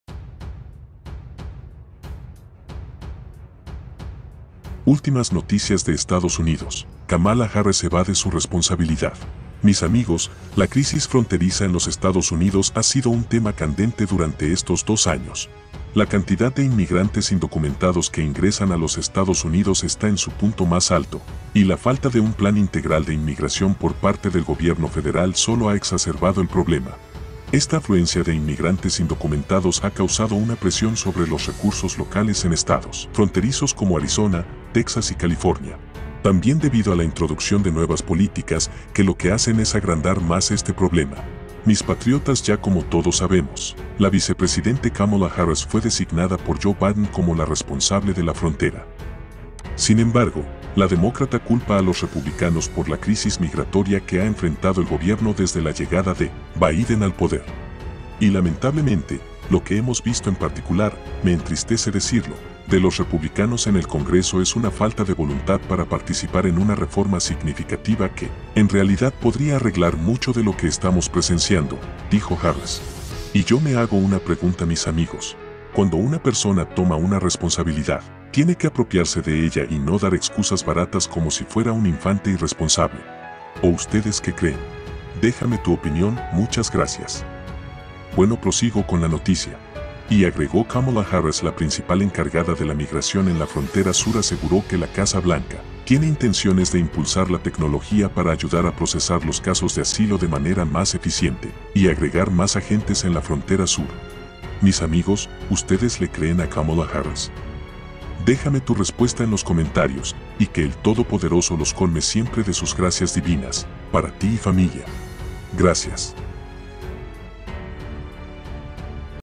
🚨🚨 ULTIMAS NOTICIAS DE ESTADOS UNIDOS. KAMALA HARRIS CULPA A LOS REPUBLICANOS DICIEMBRE 23 DE 2022
ÚLTIMAS NOTICIAS DE ESTADOS UNIDOS. KAMALA HARRIS CULPA A LOS REPUBLICANOS
MIS AMIGOS, La crisis fronteriza en los Estados Unidos ha sido un tema candente durante ESTOS DOS años. La cantidad de inmigrantes indocumentados que ingresan a los Estados Unidos está en su punto más alto, y la falta de un plan integral de inmigración por parte del gobierno federal solo ha exacerbado el problema. Esta afluencia de inmigrantes indocumentados ha causado una presión sobre los recursos locales en estados fronterizos como Arizona, Texas y California. El impacto económico de la crisis fronteriza también es de largo alcance. TAMBIEN DEBIDO A La introducción de nuevas políticas, QUE LO QUE HACEN ES AGRANDAR MAS ESTE PROBLEMA.
MIS PATROPTAS, YA COMO TODOS SABEMOS, La vicepresidente Kamala Harris fue designada por Joe Biden como la responsable de la frontera. Sin embargo, la demócrata culpa a los republicanos por la crisis migratoria que ha enfrentado el Gobierno desde la llegada de Biden al poder.
Y lamentablemente, lo que hemos visto en particular, me entristece decirlo, de los republicanos en el Congreso es una falta de voluntad para participar en una reforma significativa que en realidad podría arreglar mucho de lo que estamos presenciando, dijo Harris. Y YO ME HAGO UNA PREGUNTA MIS AMIGOS. CUANDO UNA PERSONA TOMA UNA RESPONSABILIDAD, TIENE QUE APROPIARSE DE ELLA Y NO DAR EXCUSAS VARATAS COMO SI FUERA UN INFANTE IRRESPONSABLE.
O USTEDES QUE CREEN. DEJAME TU OPINION, GRACIAS. BUENO PROSIGO CON LA NOTICIA. Y AGREGO Kamala Harris la principal encargada de la migración en la frontera sur aseguró que la Casa Blanca tiene intenciones de impulsar la tecnología para ayudar a procesar los casos de asilo de manera más eficiente y agregar más agentes en la frontera sur.
La reforma de nuestro sistema de inmigración solo puede ocurrir a través del Congreso en términos de la aprobación de un proyecto de ley de inmigración que permita un camino legal hacia la ciudadanía y una presencia legal en el país, sostuvo.
MIS AMIGOS, USTEDES LE CREEN A Kamala Harris? DEJAME TU RESPUESTA EN LOS COMENTARIOS, Y QUE EL TODO PODEROSO LOS COLME SIEMPRE DE SUS GRACIAS DIVINAS, PARA TI Y FAMILIA.